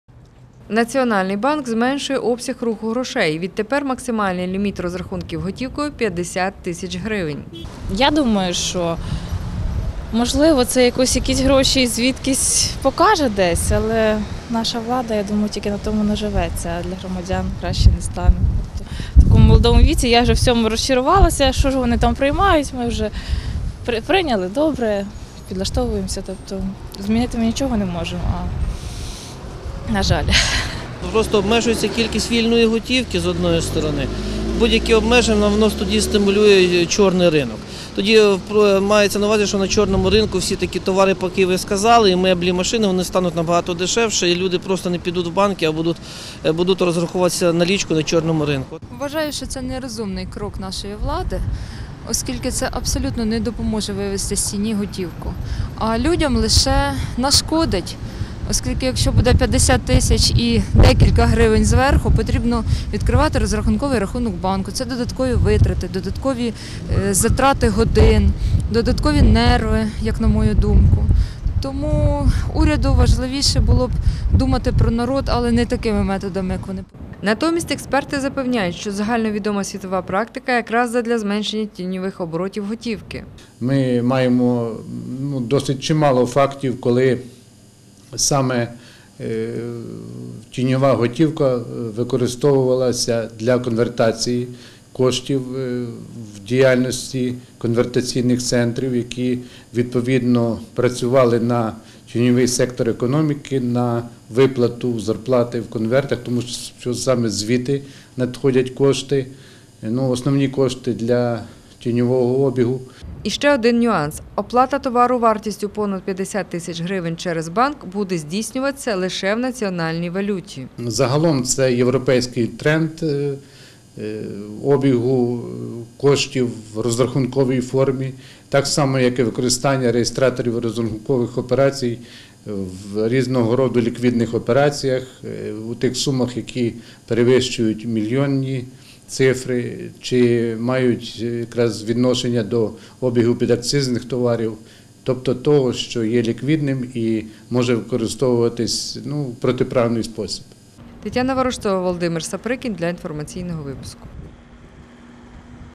Відтепер 50 тисяч гривень – гранична сума розрахунків готівкою
Купувати  дороговартісні товари за готівку відтепер буде не просто. Якщо ви надумали придбати автомобіль, квартиру, коштовні прикраси,  дорогий одяг  чи  дорогу техніку, і  вартість покупки складає більше 50 тисяч гривень, то вам доведеться розраховуватись,  перераховуючи кошти на рахунок. Інакше до вас завітають гості з фіскальної служби.